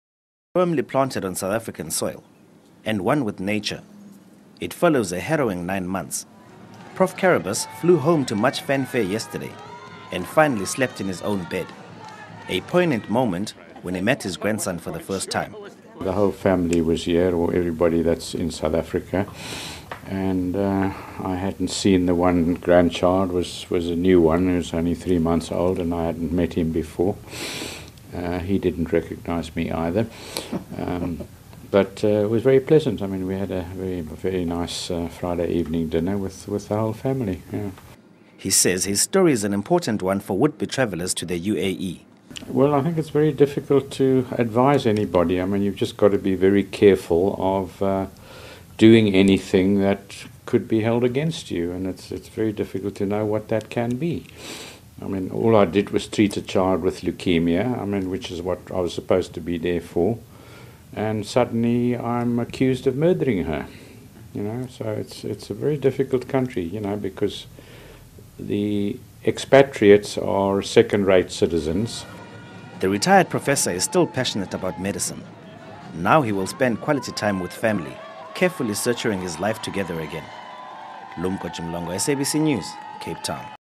Professor Cyril Karabus, has warned South Africans working or planning to work in the UAE.
Cape Town paediatric oncologist, Professor Cyril Karabus,
has warned South Africans working or planning to work in the UAE, to exercise extreme caution.
Karabus spoke to the SABC at his Cape Town home -  having spent the first night back with his family.
He had been detained in Abu Dhabi in August last year after being convicted in absentia for manslaughter, relating to the death of a girl he treated for leukemia in 2002.